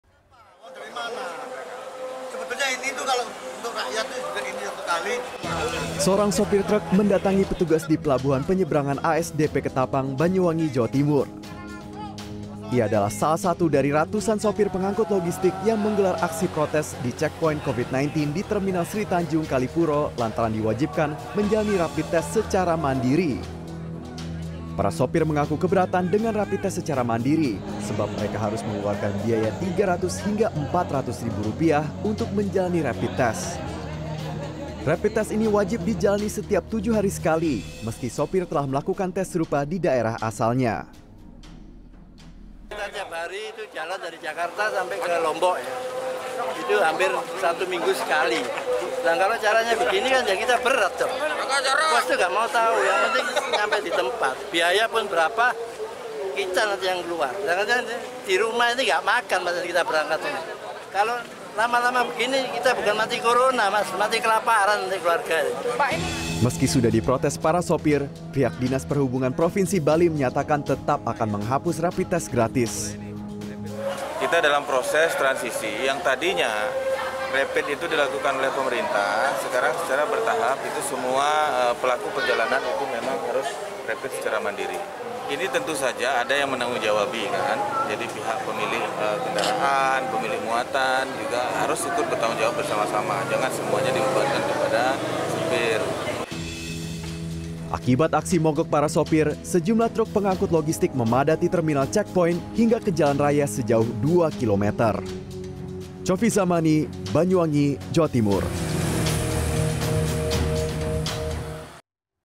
Wajib Rapid Test Berbayar, Sopir Truk Protes
Ratusan sopir truk ekspedisi pengangkut logistik, menggelar aksi mogok, di check point Covid-19, di terminal Sritanjung, kecamatan Kalipuro, Banyuwangi, Kamis siang. Aksi ini dilakukan, karena pemerintah provinsi Bali meniadakan rapid test gratis, dan mewajibkan sopir truk menjalani tes cepat secara mandiri, sebelum menyeberang ke pulau Bali.

Like & Follow Facebook:
CNN Indonesia

cnnindonesiatv